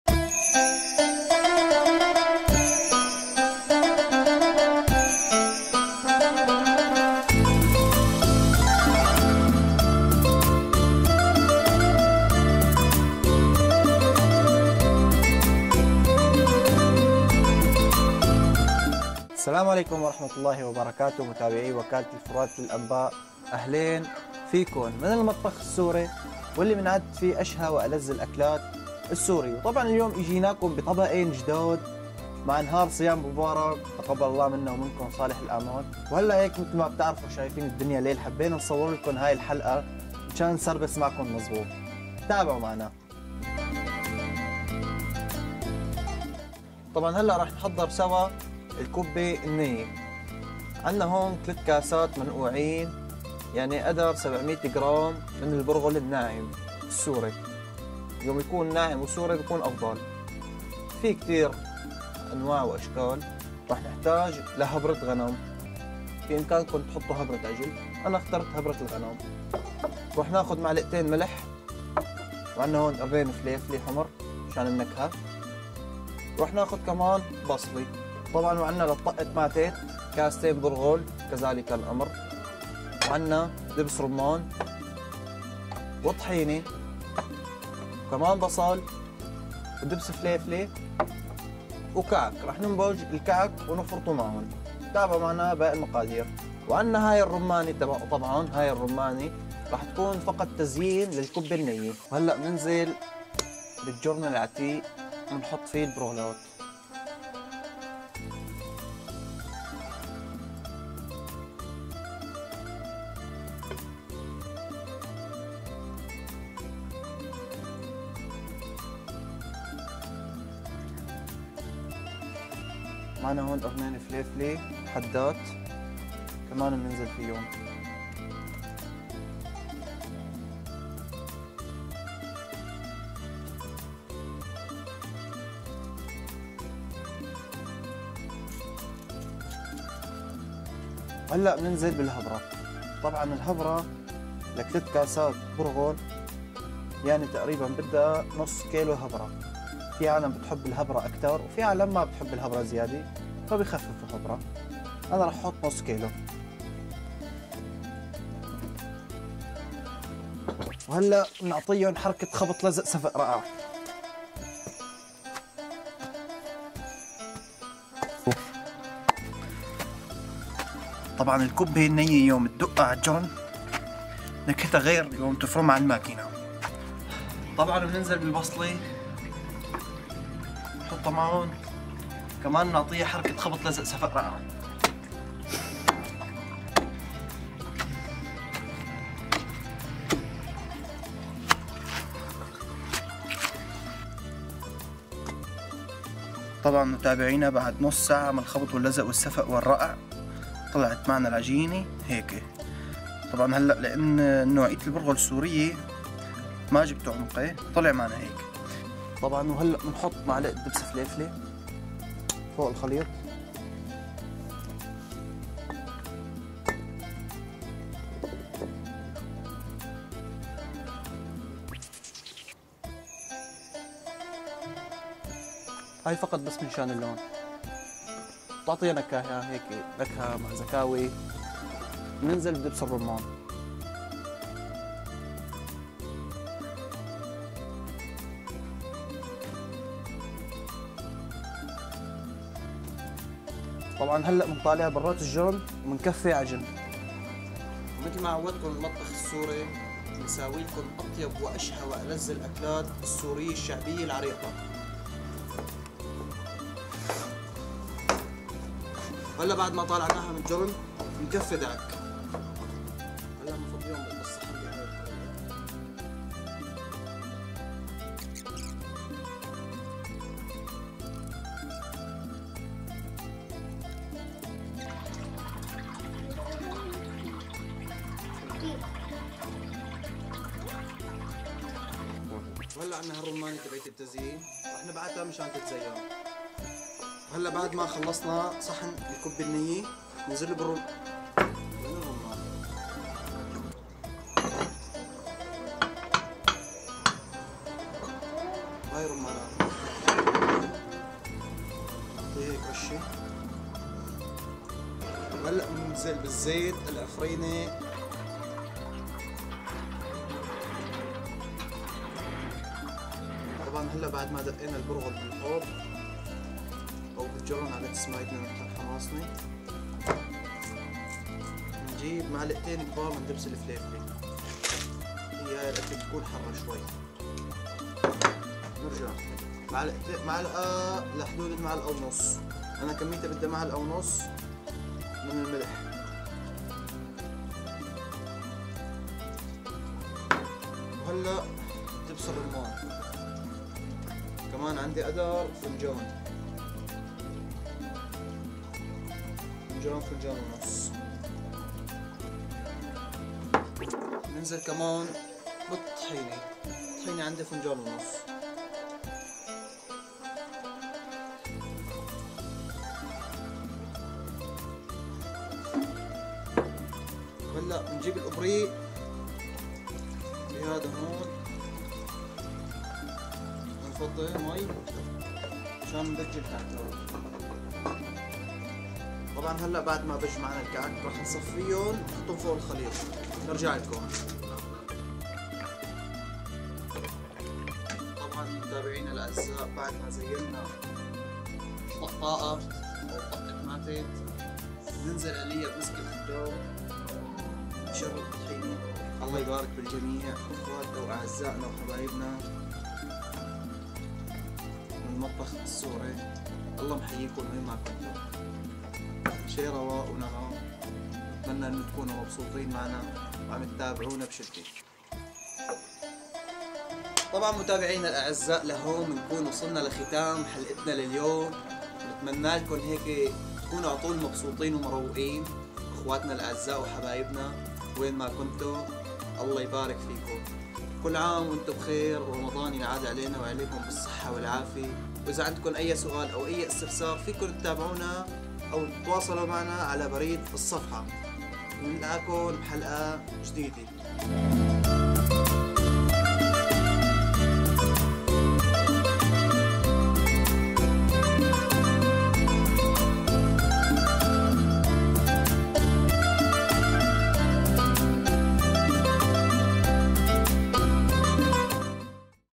المطبخ السوري.. أشهر الوصفات الرمضانية السورية | طبق اليوم: كبة نية وطقت ماتت (طقاقة)
المطبخ السوري.. أشهر الوصفات الرمضانية السورية
يُعد المطبخ السوري من أغنى وأعرق المطابخ العربية، فهو نتاج ثقافات وحضارات متعددة امتزجت على مر العصور، ويشتهر بتنوعه وثرائه، حيث يضم مجموعة واسعة من الأكلات التي ترضي جميع الأذواق.
طبق اليوم: كبة نية وطقت ماتت (طقاقة)